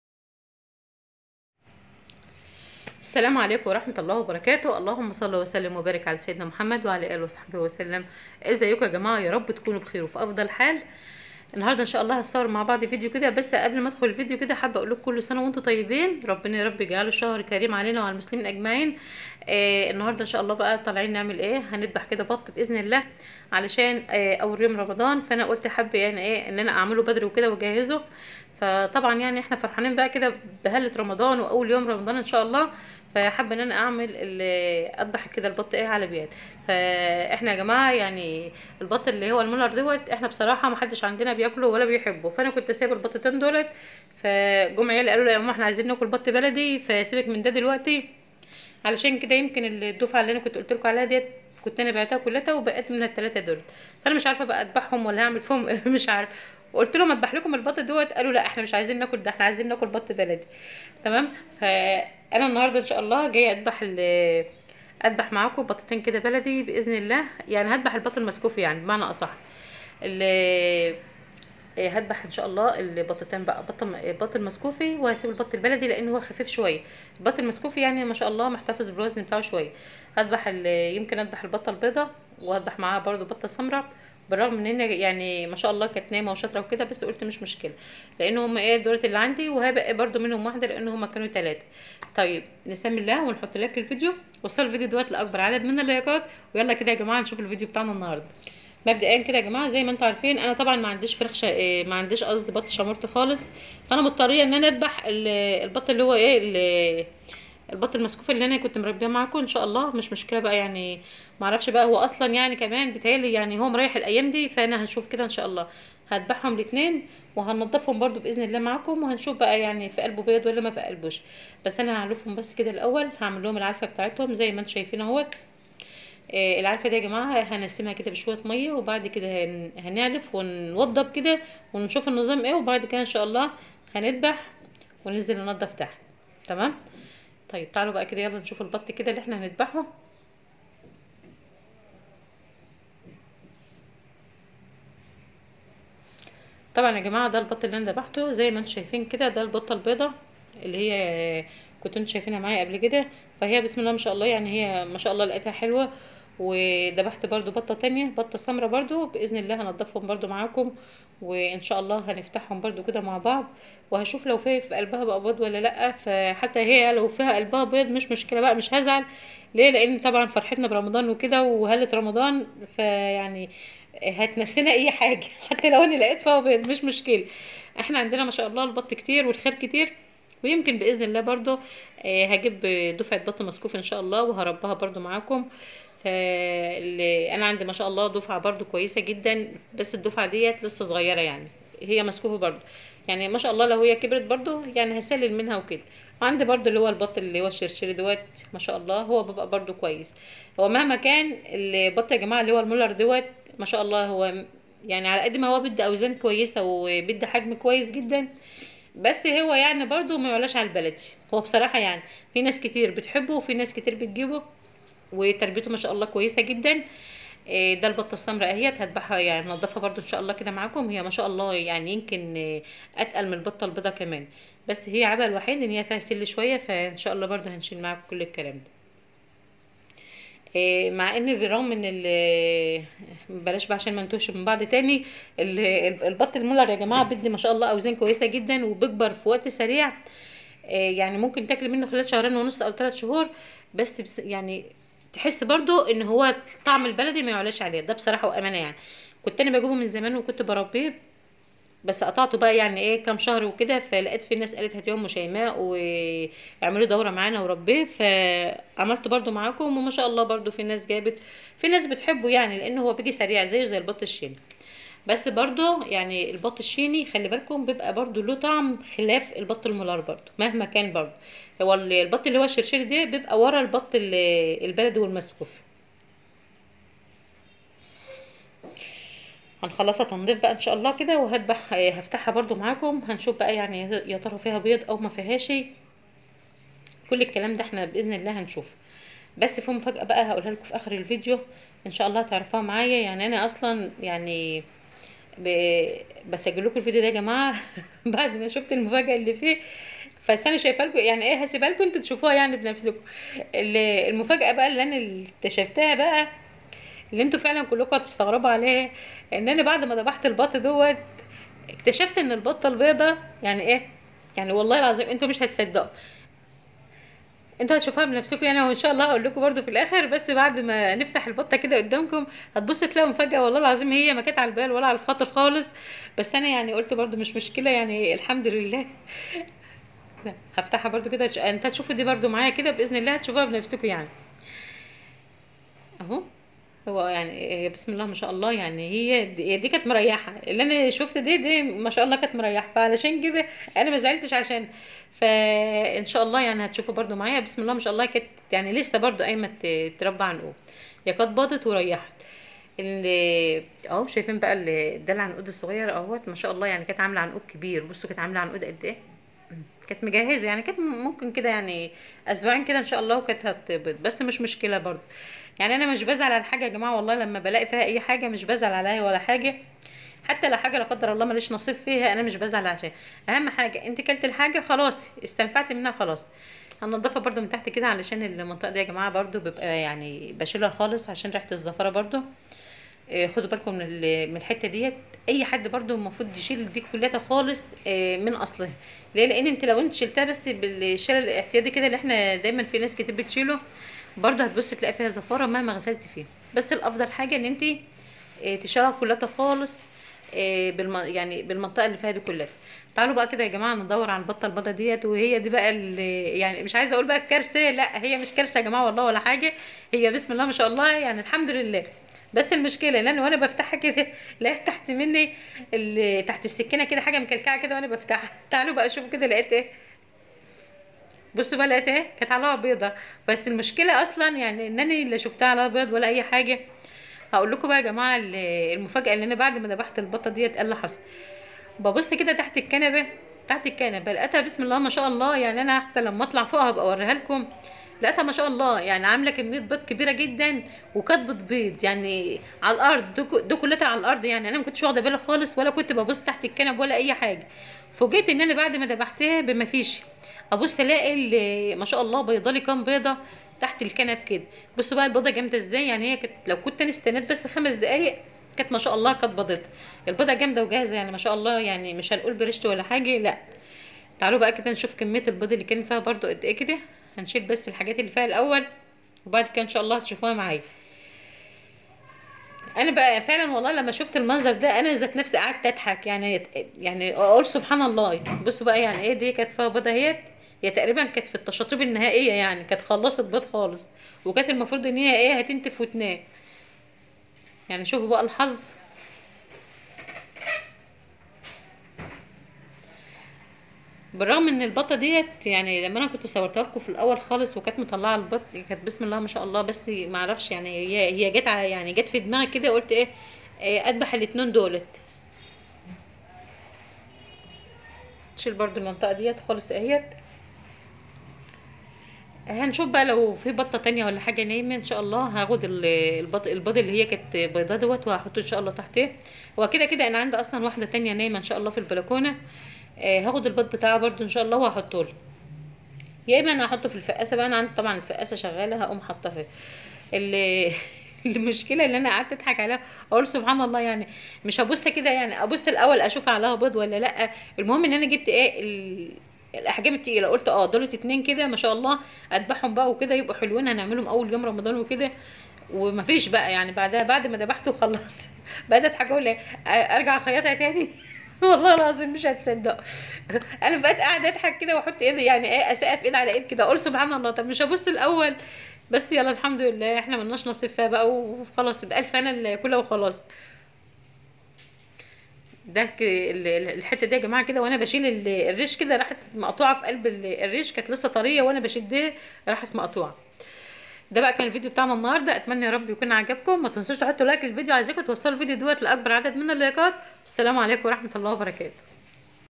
اتصدمت من اللى شوفته فى البطة ويارتنى سبتها بعد اللى لقيته فيها مفاجأة صدمتنى وخلتنى مزهولة
اتصدمت من اللى شوفته فى البطة البيضاء ويارتنى سبتها بعد اللى لقيته  فيها مفاجأة صدمتنى وخلتنى مزهولة